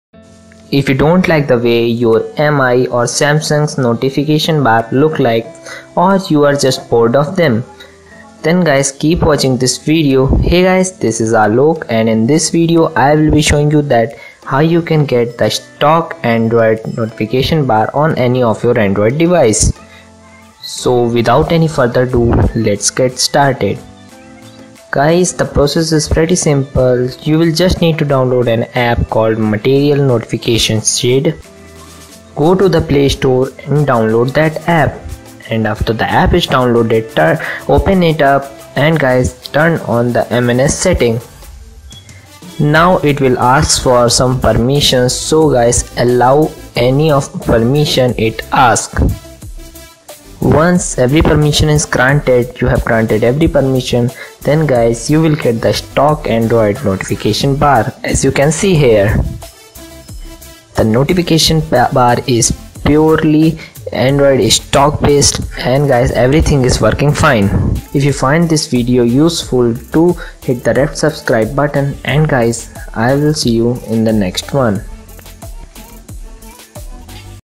How to get stock android notification bar [English]
Hi guys,
In this video i have shown you that how you can get stock android notification bar on any of android device.
L.C.S.S
L-Like
C-Comment
S-Share